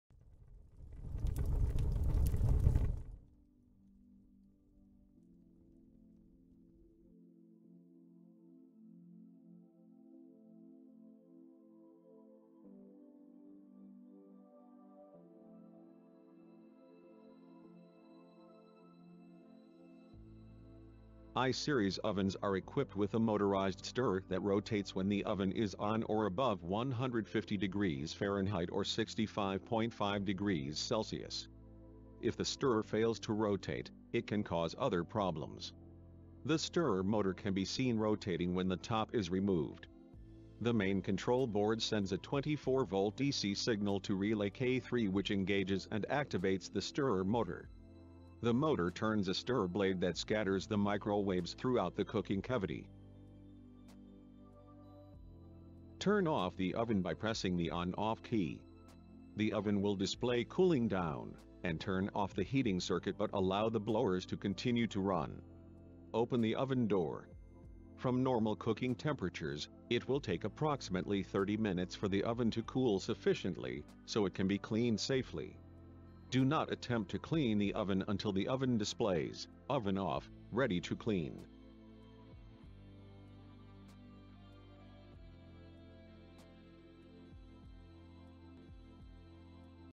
TurboChef iSeries Demo
This course provides both the basic principles and comprehensive service training for the TurboChef iSeries Rapid-Cook Oven. It covers fundamental concepts such as installation, sequence of operations, troubleshooting, and maintenance.